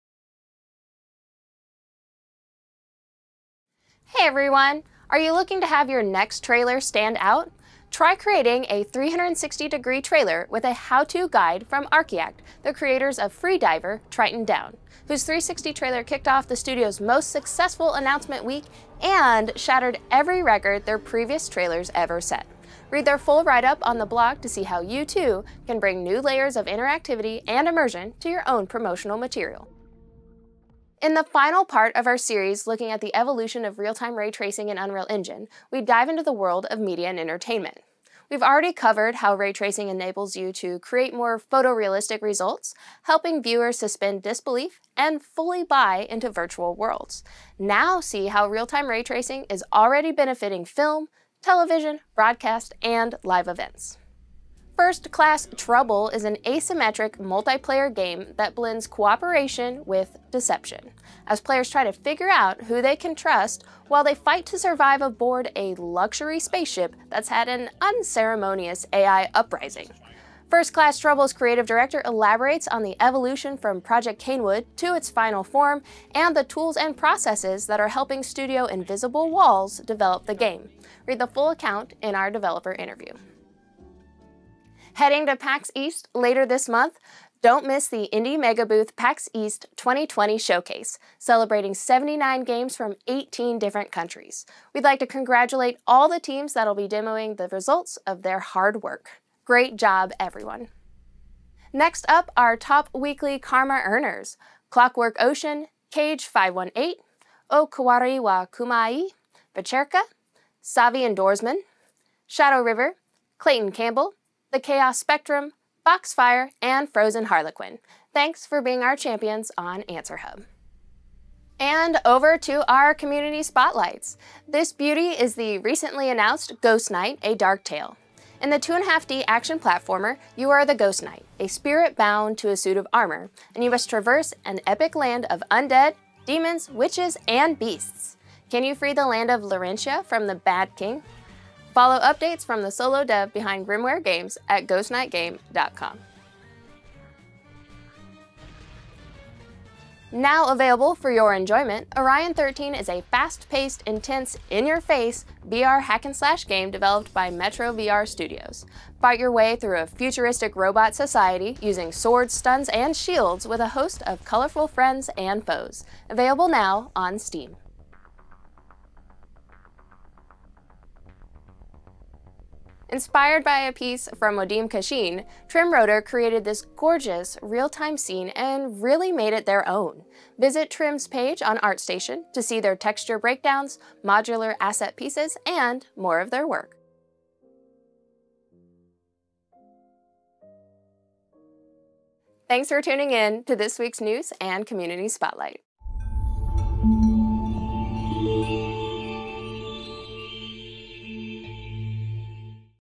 News and Community Spotlight | February 13, 2020 | Unreal Engine
NEWS

A comprehensive guide to creating 360-degree game trailers using Unreal

Real-time ray tracing in Unreal Engine - Part 4: media and entertainment

Developer Invisible Walls creates asymmetrical multiplayer game with no previous Unreal experience

COMMUNITY SPOTLIGHT